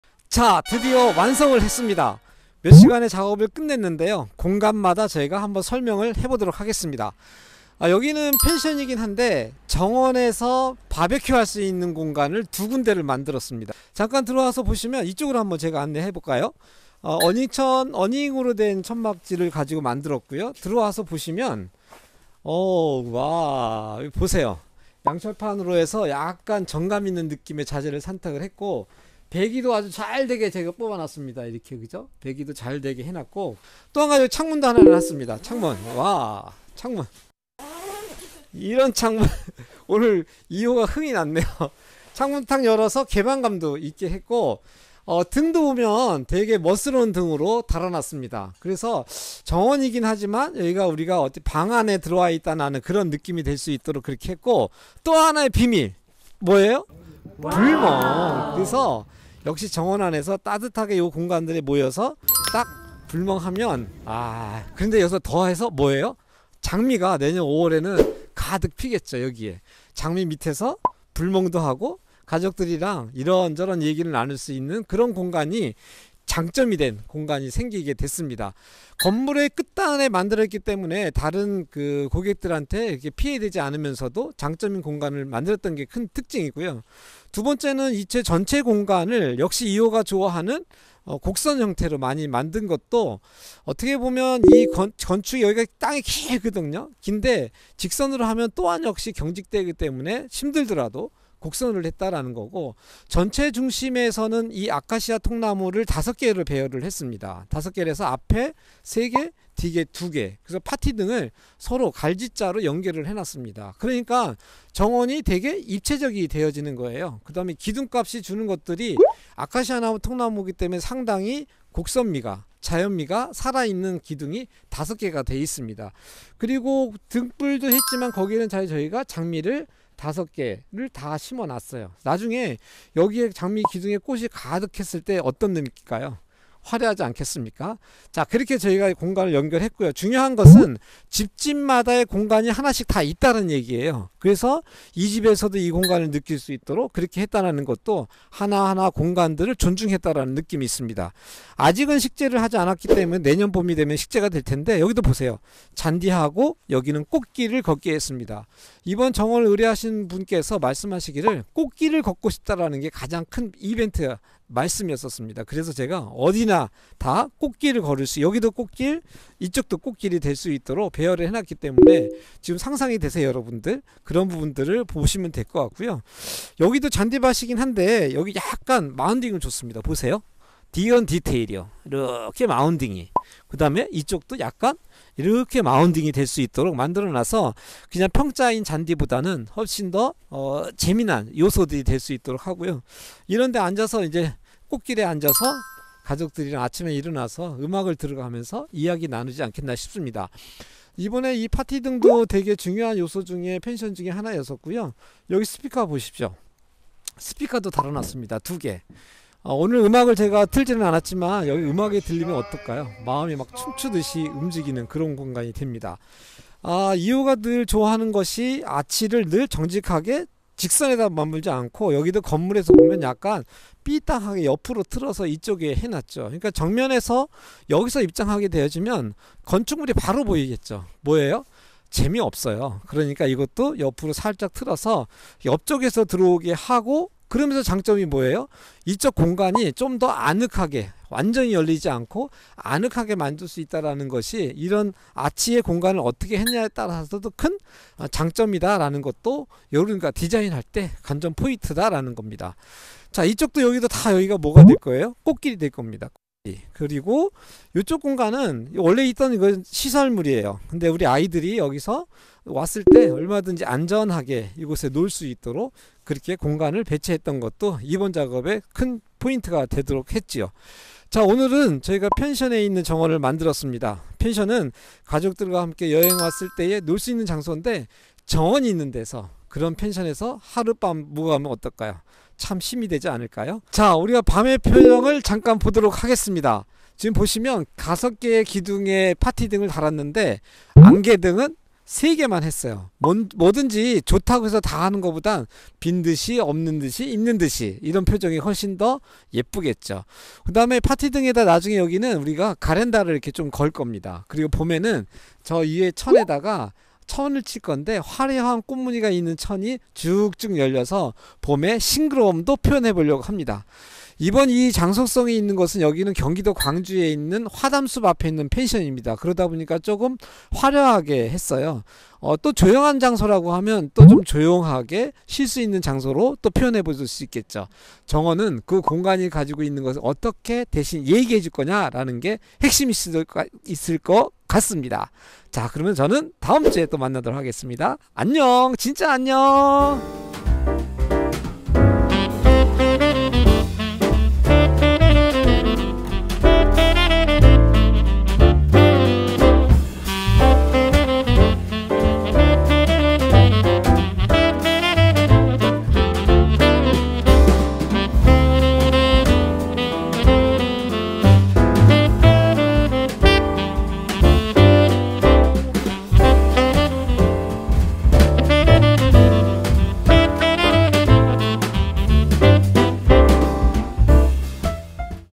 그냥 지나칠 수 없는 펜션 정원 완성 컷 | 불명&정원 바베큐의 힐링 | 정원 시공 영상 | 곤지암 로뎀 나무 펜션
로뎀나무 펜션 홈페이지는 아래 링크로 들어가실 수 있어요~

🌹네이버 밴드 - 이오와 정원친구들이 서로의 이야기를 나누는 공간